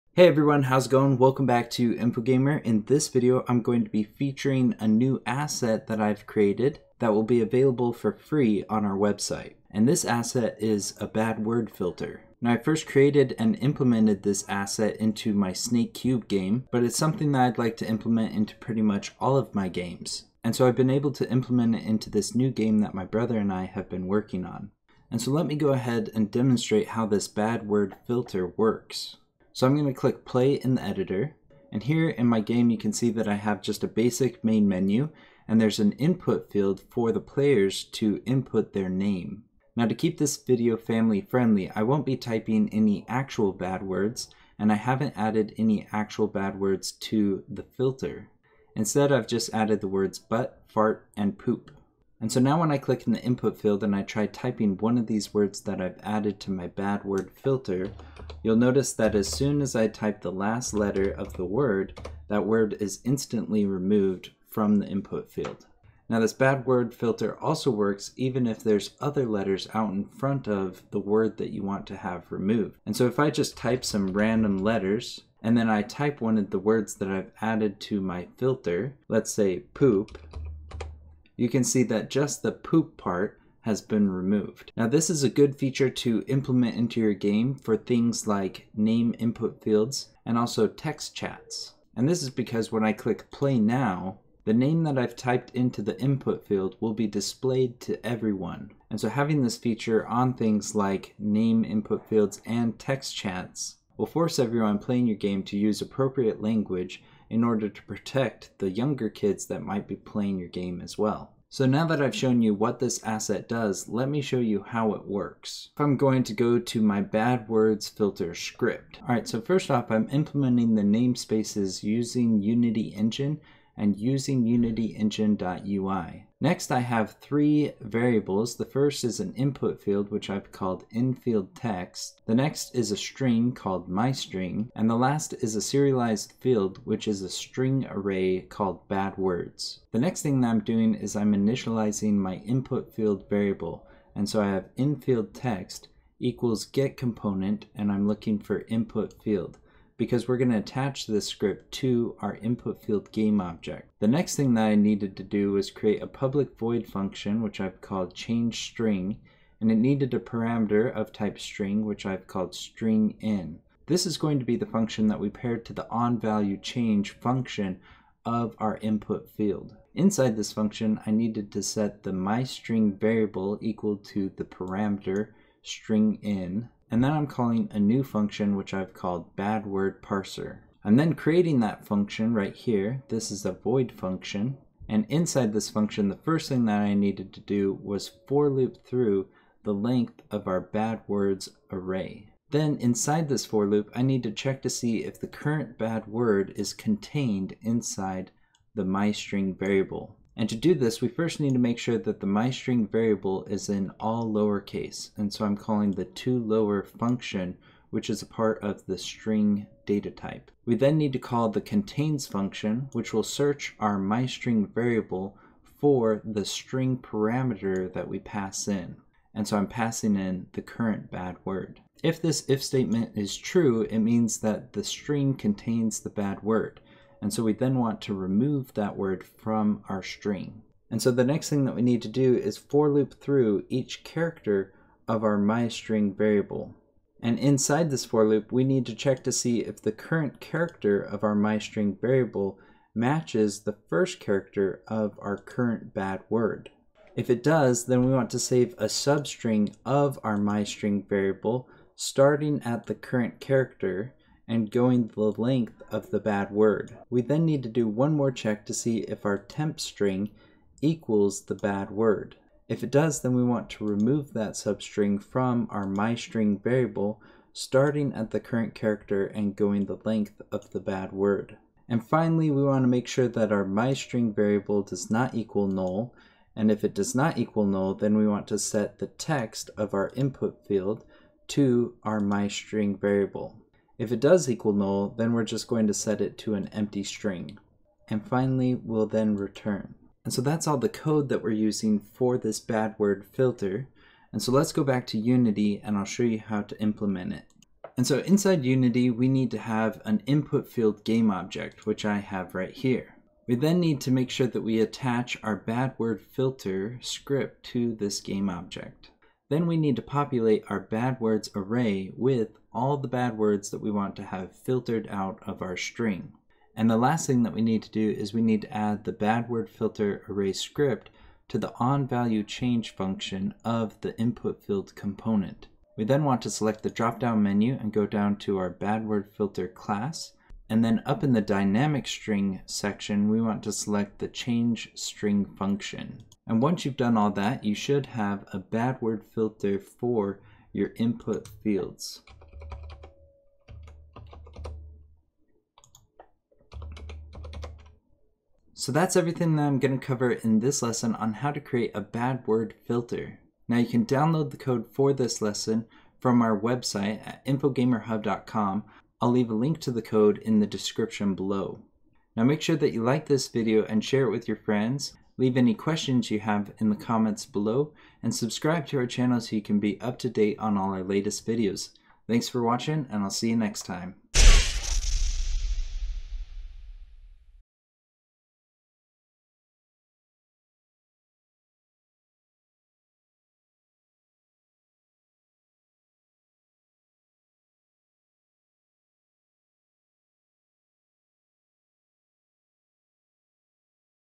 Bad Word Filter Asset for Unity
This is our bad word filter asset for Unity that will clean any input field from bad words that you do not want in your game. This script will parse your input string and remove any of the bad words you add to the filter. You can download this script for free from the asset store on our website.